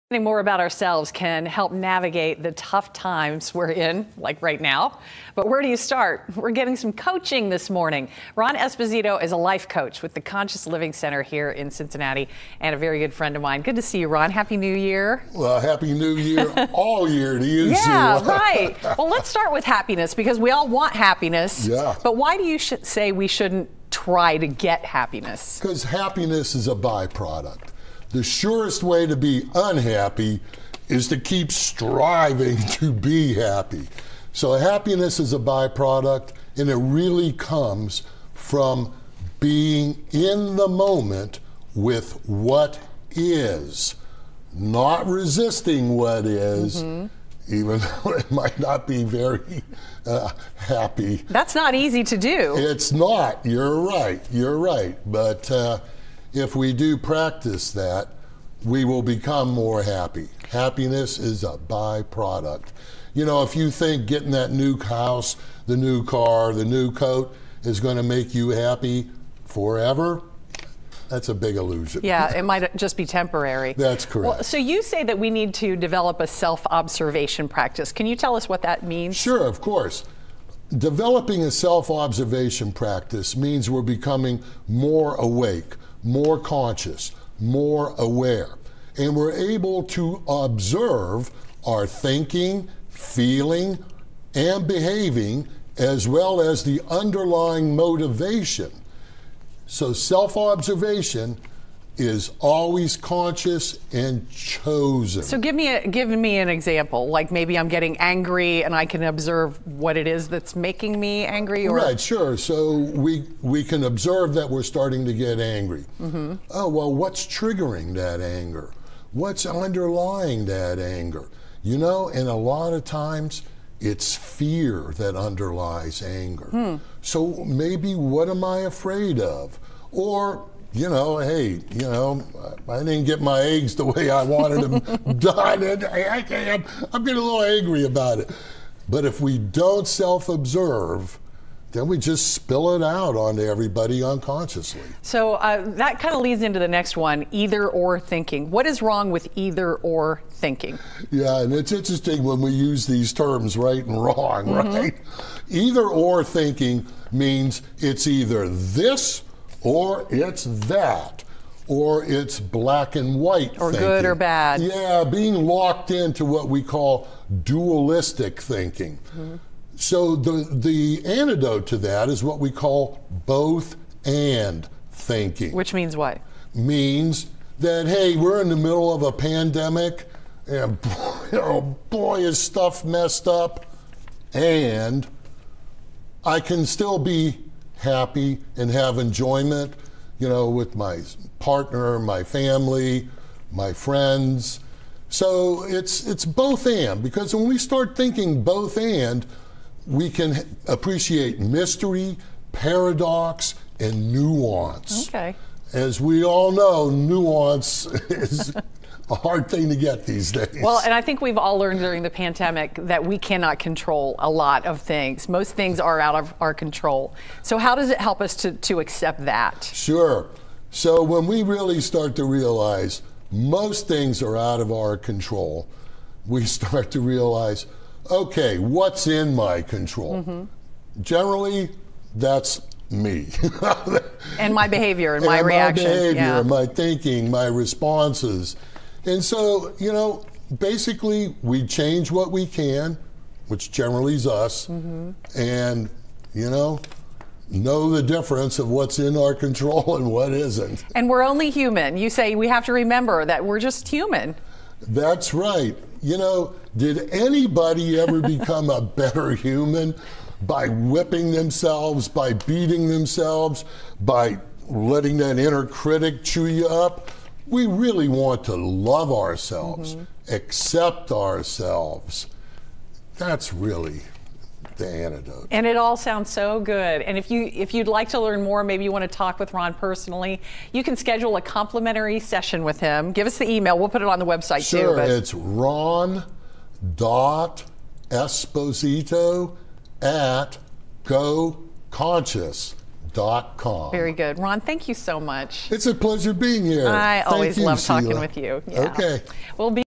Life coach has advice for navigating tough times
Understanding more about ourselves can help navigate the tough times like we're in now